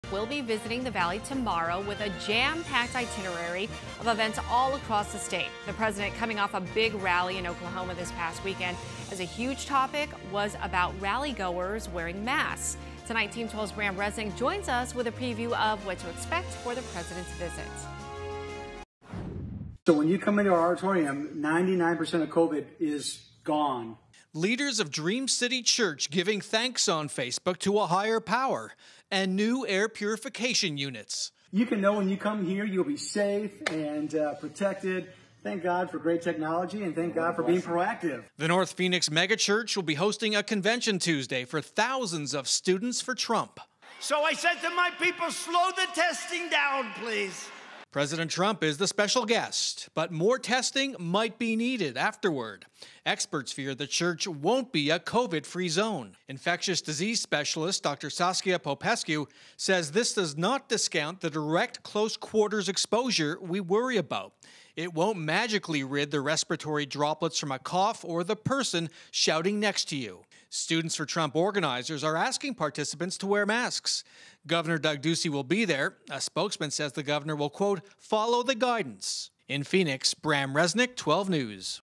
Here’s what you can expect from President Trump’s Arizona visit
President Donald Trump will visit Yuma and Phoenix for a variety of events in Arizona, according to a White House official.